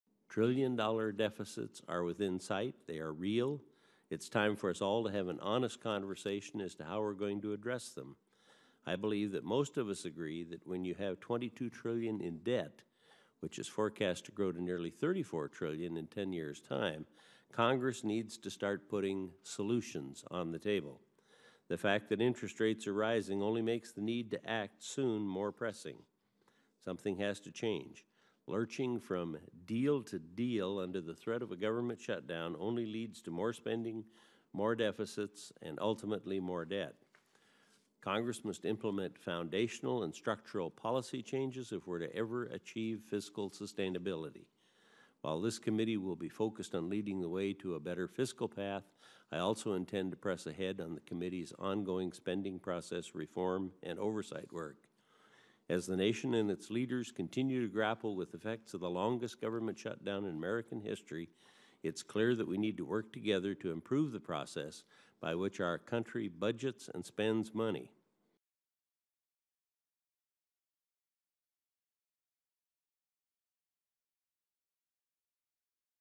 Enzi: CBO Report Highlights Need for Congress to Confront Out of Control Spending
During a Senate Budget Committee hearing on the Congressional Budget Office’s Budget and Economic Outlook for fiscal years 2019 through 2029, Chairman Mike Enzi (R-WY) said the CBO report highlights the need for Congress to confront the threat of the nation’s out of control spending.